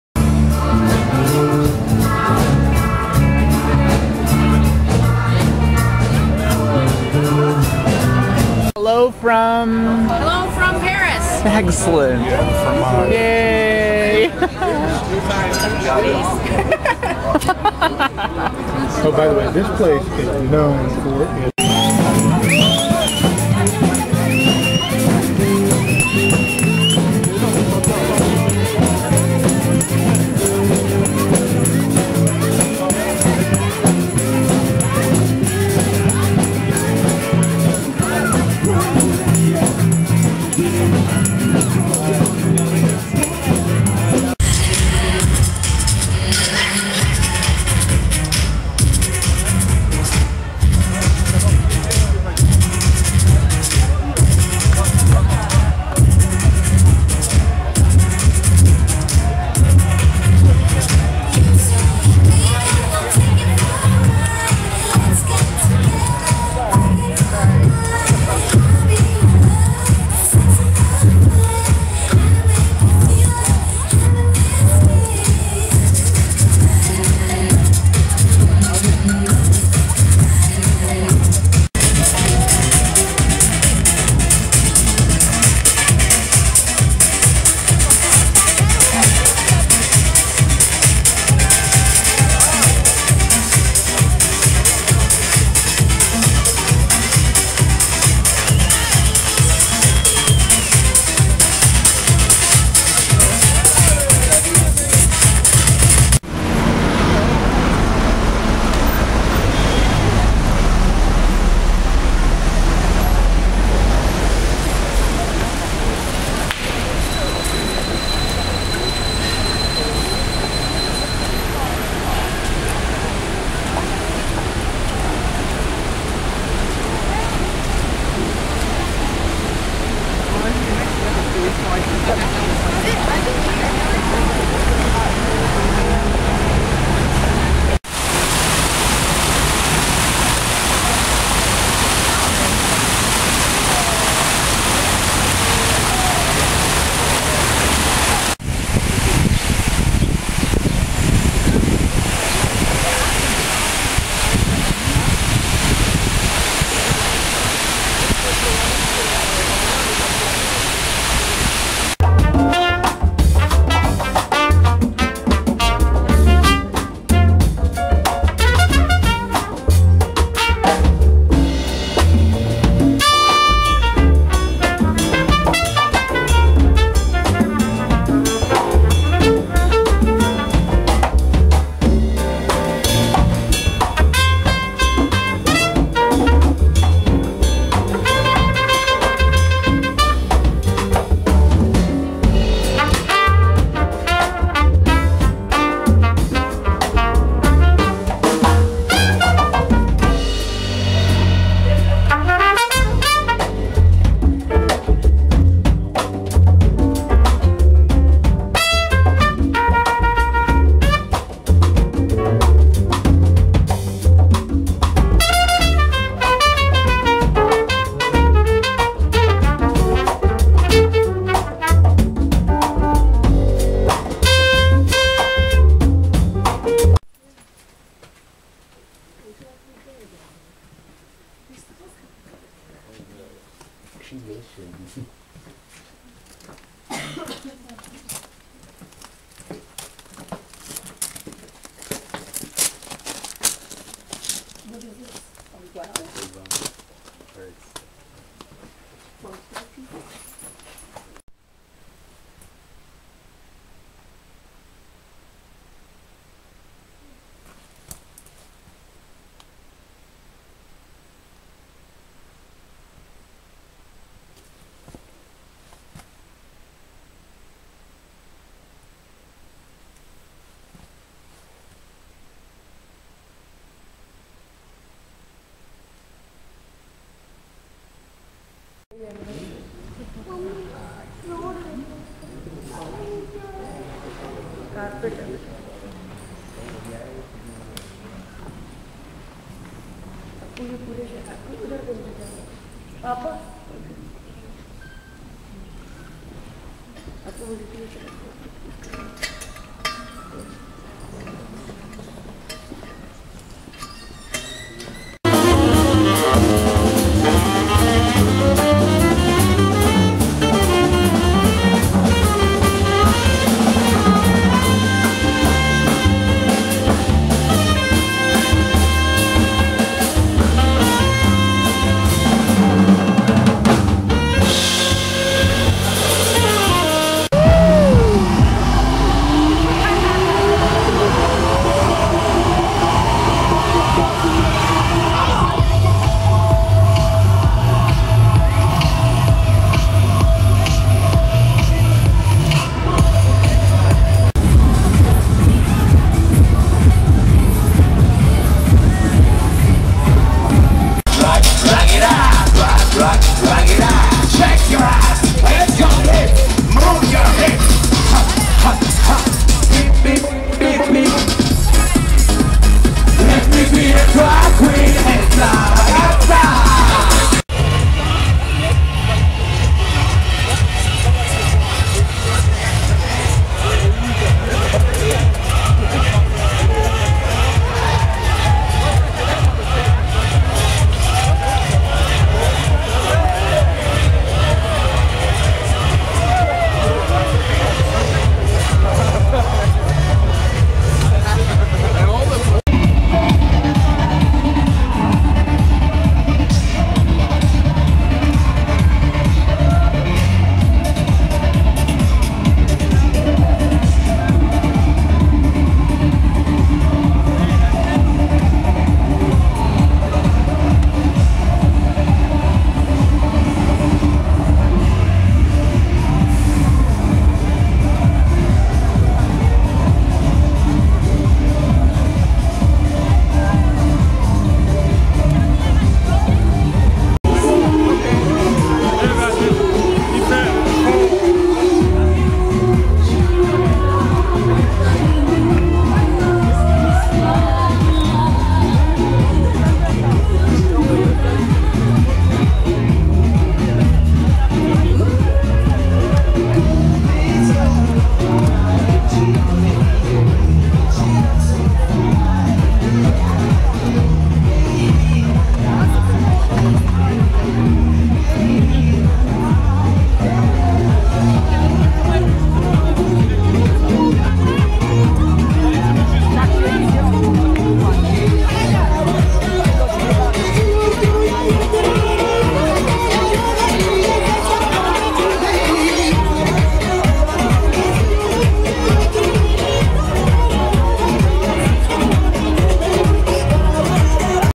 Paris June 2010 part 2 music, street scenes,catacombs, gay pride
music, street scenes,catacombs, gay pride